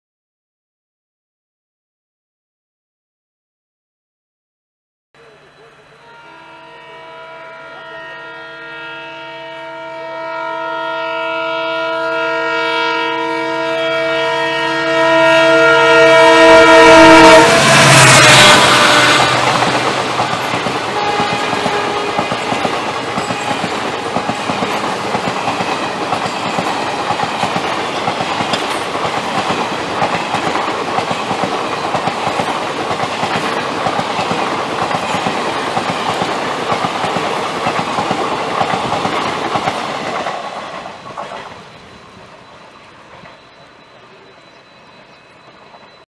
Netravati Express overtaking MAO-LTT Winter Special @ Kolad
16346 TVC-LTT Netravati Express headed by WDP-4D #40083 overtaking our 01230 MAO-LTT Winter Special at Kolad station on Konkan Railway. Lovely honking by the Netravati LPs.